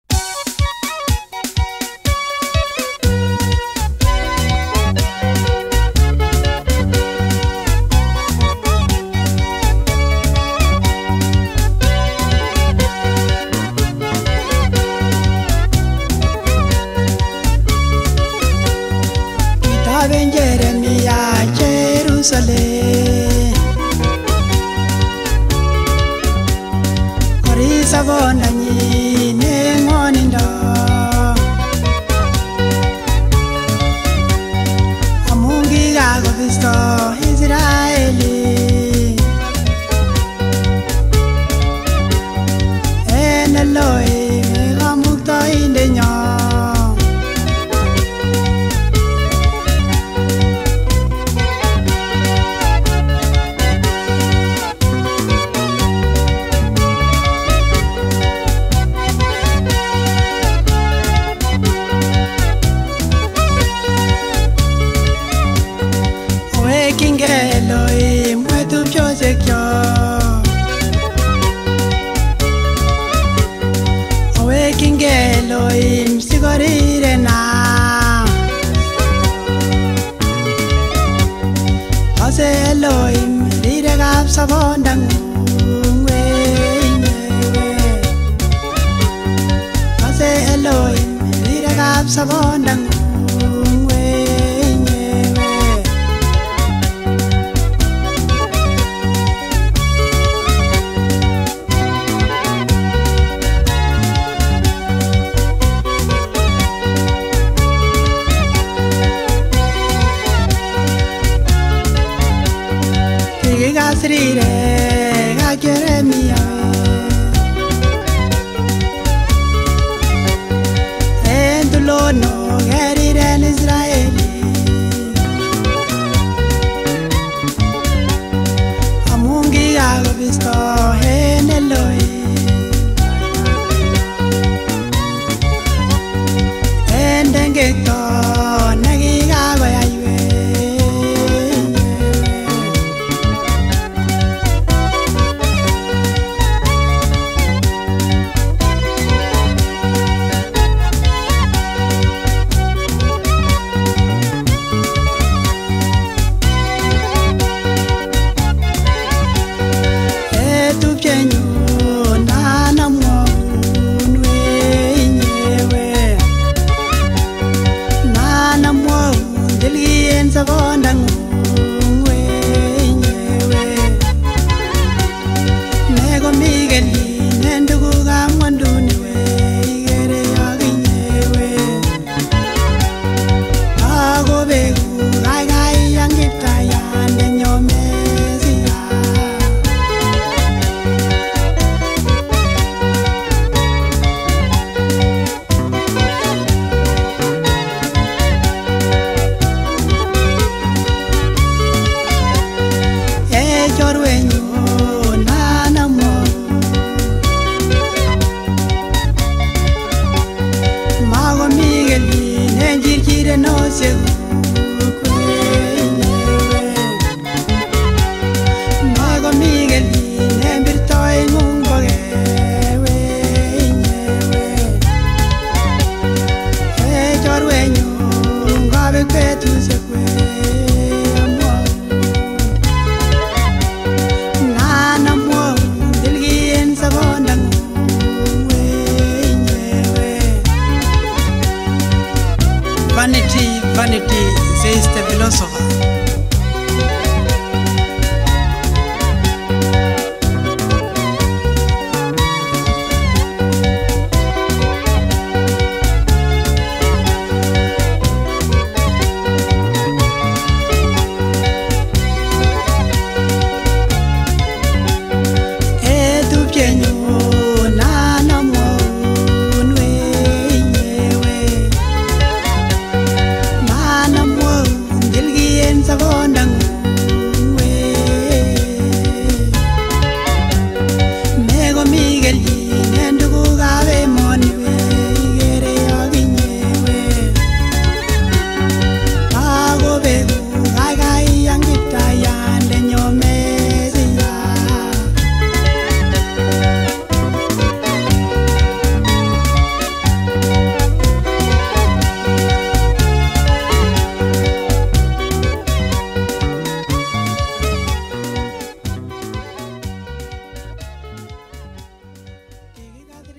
Kitoben Jeremiah Jerusalem by Pastor Paul Ngeno
The prophet Jeremiah wept over the city of Jerusalem due to it's filth